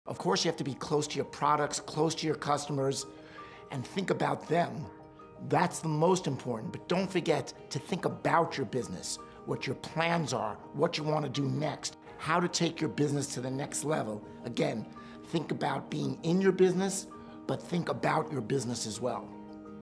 Lloyd Blankfein's advice for entrepreneurs - Clip
Lloyd Craig Blankfein is an American investment banker who has served as senior chairman of Goldman Sachs since 2019, and chairman and chief executive from 2006 until the end of 2018